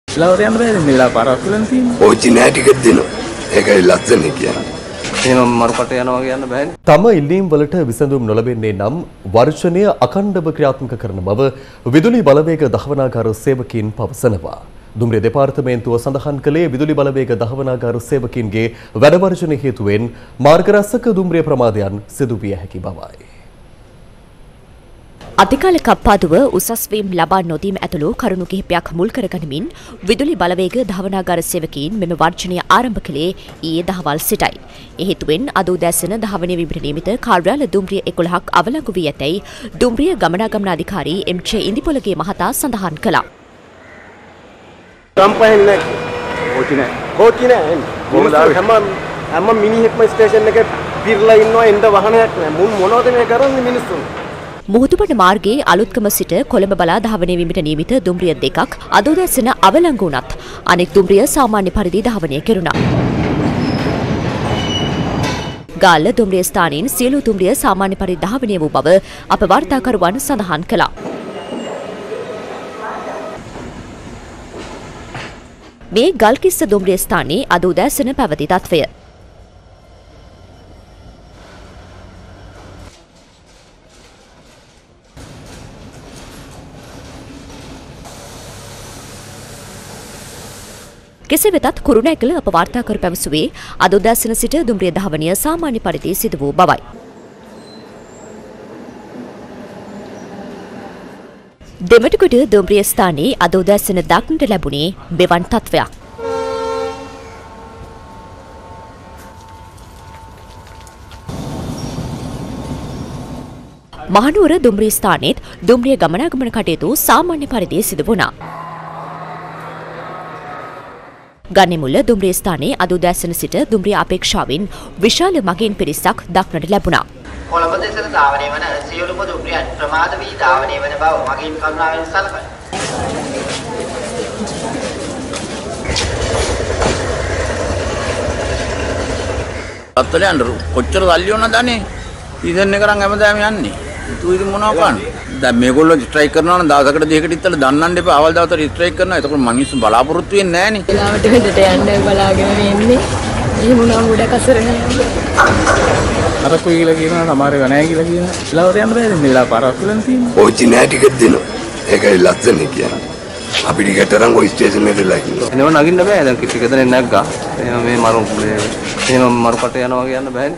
විසඳුම් නැත්නම් වැඩ වර්ජනය දිගටම..-දුම්රිය විදුලි බලවේග ධාවනාගාර සේවකයින්-
🔺"මෙහෙම මරු කටේ යනවා වගේ නගින්න බැහැනේ"